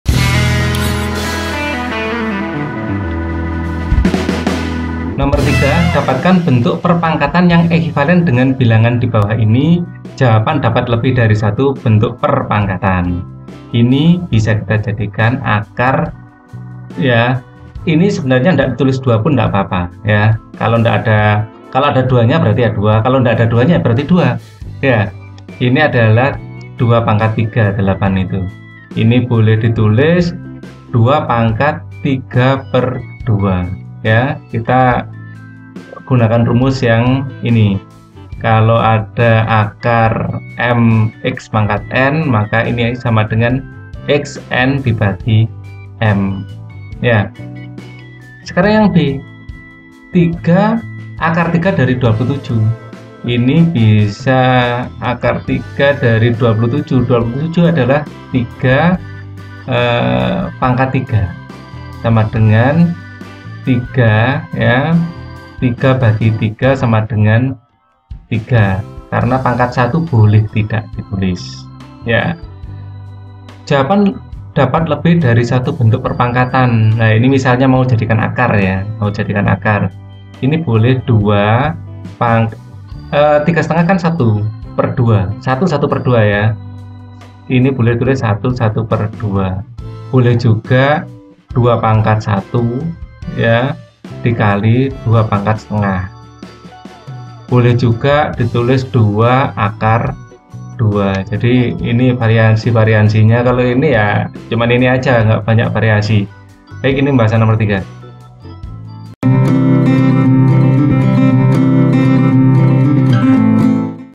merubah bentuk akar ke perpangkatan, bse matematika kelas 9 ,k13 uk1 no 03 , eksponensial
dapatkan bentuk perpangkaran yang ekivalen dengan boangan dibawah ini